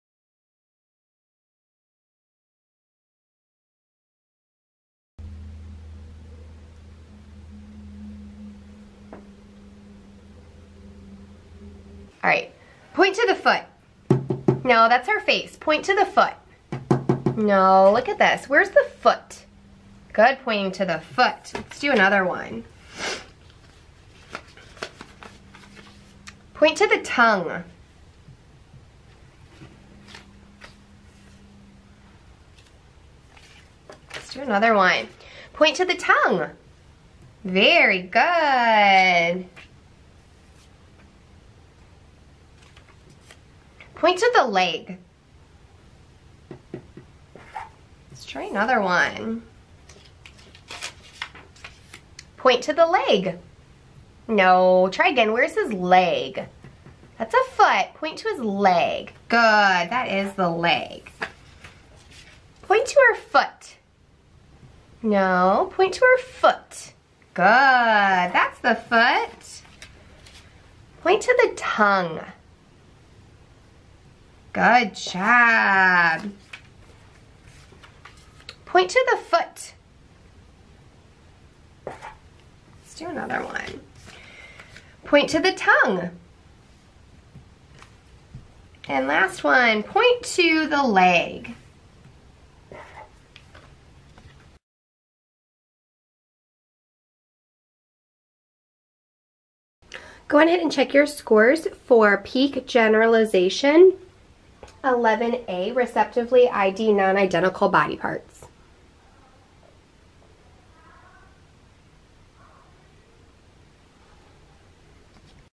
PEAKG; 11A Demo
PEAK-Generalization; Receptively ID Non-Identical Body Parts - 11A demonstration and scoring.